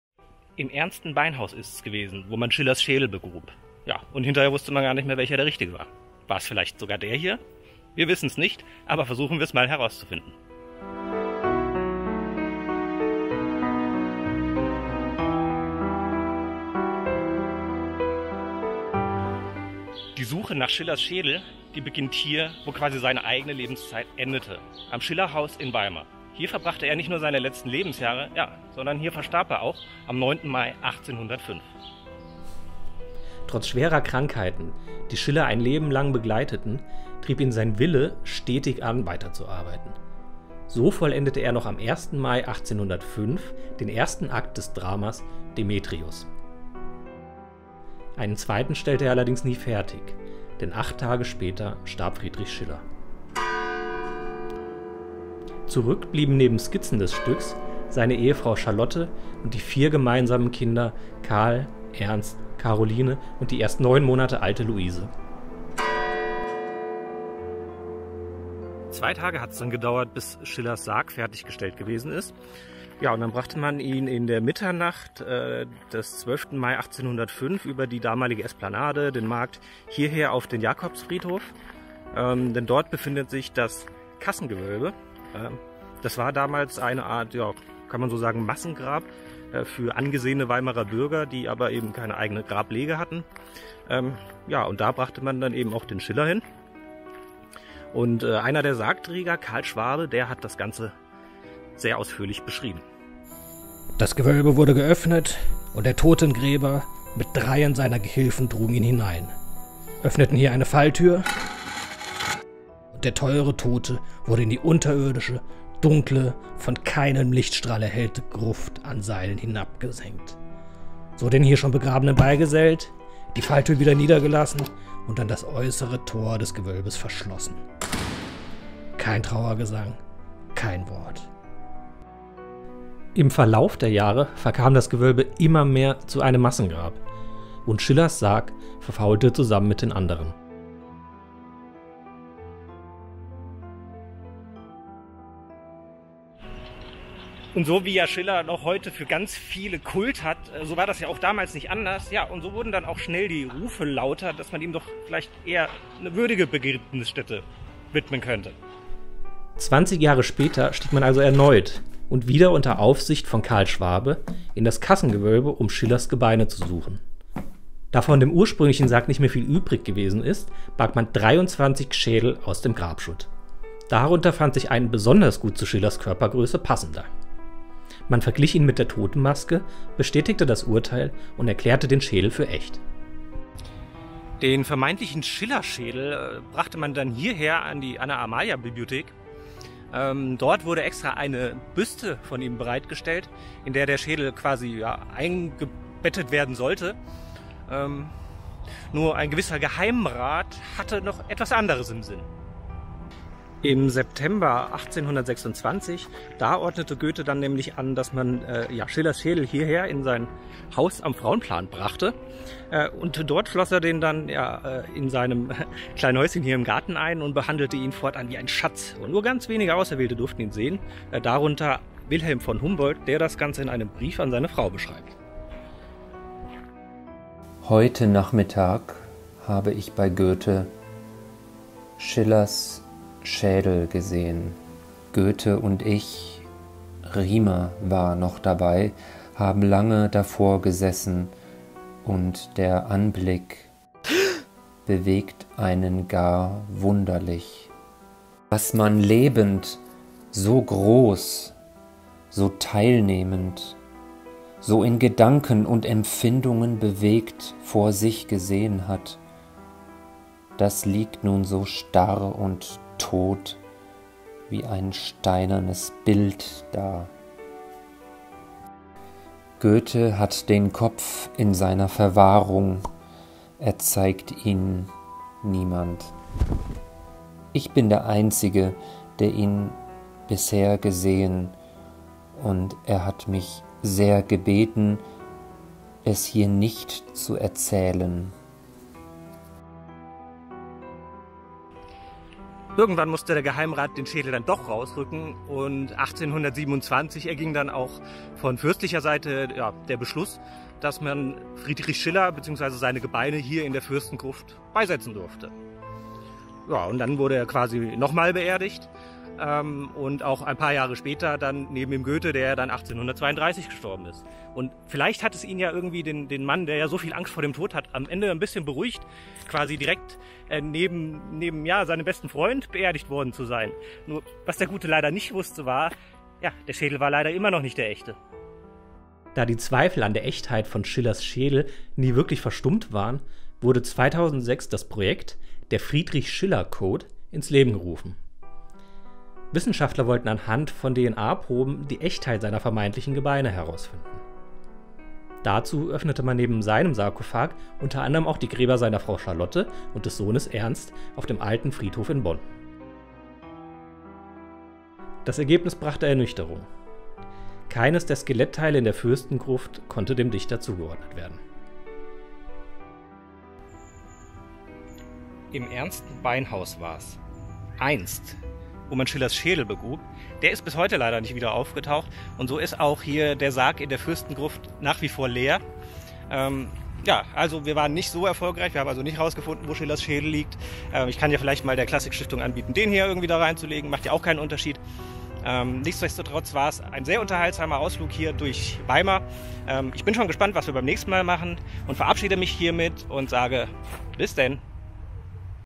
Goethe und Schillers Schädel
Wieso man 20 Jahre nach Schillers Beerdigung seinen Schädel suchte, was Goethe damit vorhatte und warum Humboldt niemandem davon erzählen durfte – Darum geht es in diesem Video.

Humboldt-Sprecher:
Damian Mallepree

Quellen:
Peter Braun: Weimarer Geschichten. Deutscher Taschenbuch Verlag, 2009.
Theodor Kappstein: Wilhelm von Humboldt im Verkehr mit seinen Freunden. Wilhelm Borngräber, ca. 1918.

Sounds:
443958__anational__box-dropping-with-stones
581545__newlocknew__walking-up-and-downstairs-wooden-stair-dns-vlshpng-eq
43553__dobroide__20071102-pealing-bell
149111__klankbeeld__deep-breath-in-male-01
352514__inspectorj__ambience-night-wildlife-a
120353__cemagar__towel
343097__edsward__plopenhanced
168821__debsound__stone-door-004
530987__patchytherat__stone-door-close
524231__sheyvan__heavy-metal-door-2
Writing_01.WAV by Leandri140029